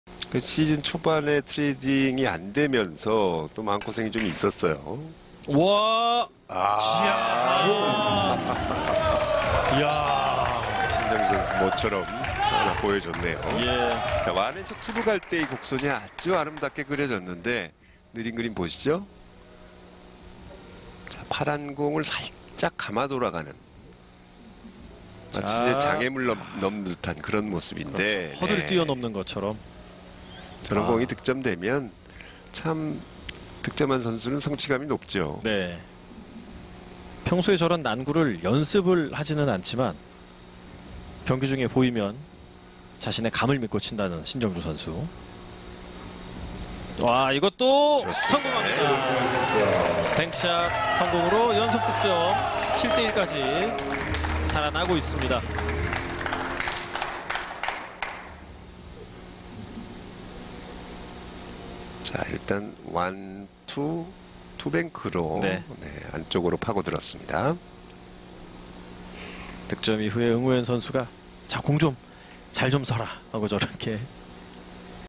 당구대에 그리는 아름다운 곡선! 신정주의 환상적인 득점![웰컴저축은행 PBA 팀리그 2023-2024] 2라운드
PBA 팀리그 2023~24시즌 2라운드 주요장면 및 하이라이트 영상